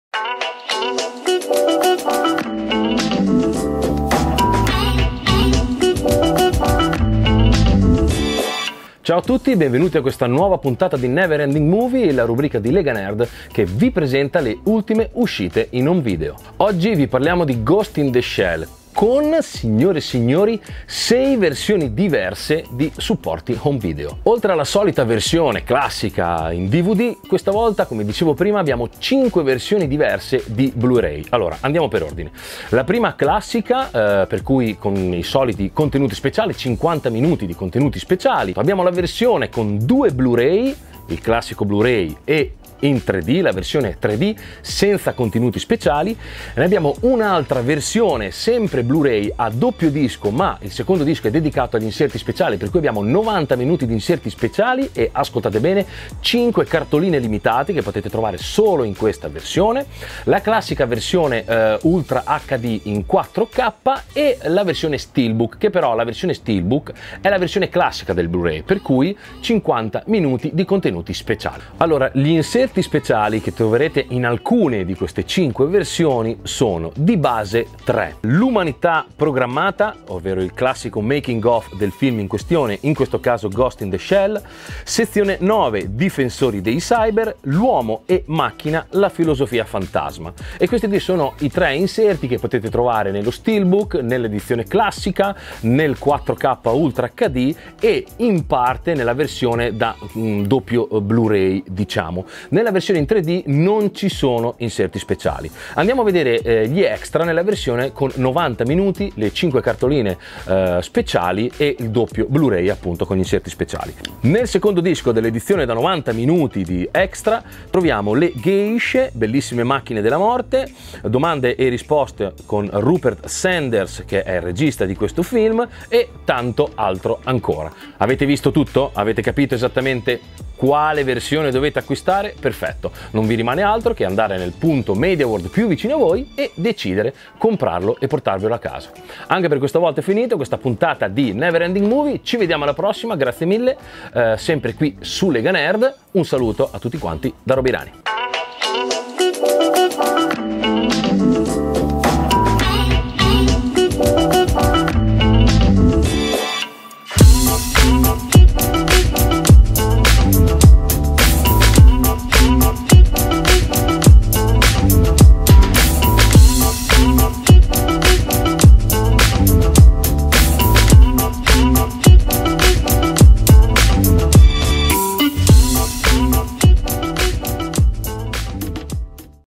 Ghost in the Shell - Edizioni DVD e Blu-Ray Home Video
Torna Never Ending Movie, la nuova rubrica di Lega Nerd​ dedicata all’Home Video: con Roby vi parliamo dei diversi formati, dei contenuti speciali e delle edizioni speciali dei film in uscita in home video. In questa puntata è il turno di Ghost in the Shell, il film di Rupert Sanders con Scarlett Johansson.